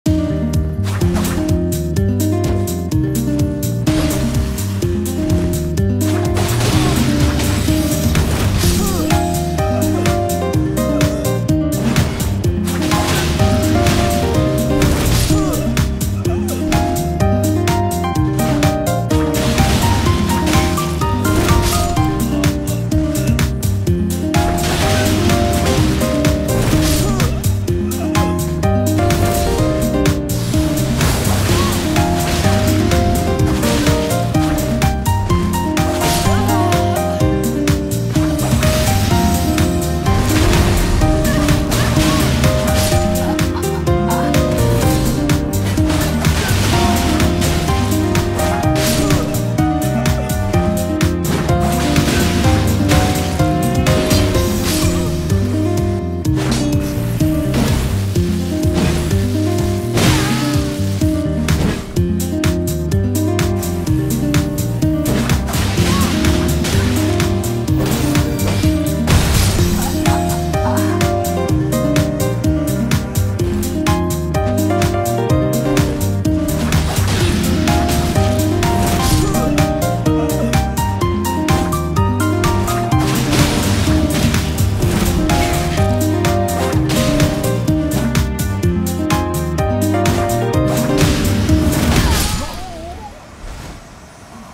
NEW 3 hit Hookclaws TRUE COMBO (with 413 INT scaling scholar buff)
But Thiccas, why the scholar buff? ...Because it looks sick on claws! Also, at 70 INT, you'll get 413 INT scaling (with the lunsat staff) giving you 301+ pure magic damage on top of some decent DEX scaling when infused with Keen. Claws also got access to impaling thrust. Impaling thrust functions really well as a roll/panic catch.

It looks cool and has some good AR? Why not? You can also go with keen and bloodflame blade with some faith for the fire damage. Might not be worth the fire damage, but the blood build is amazing.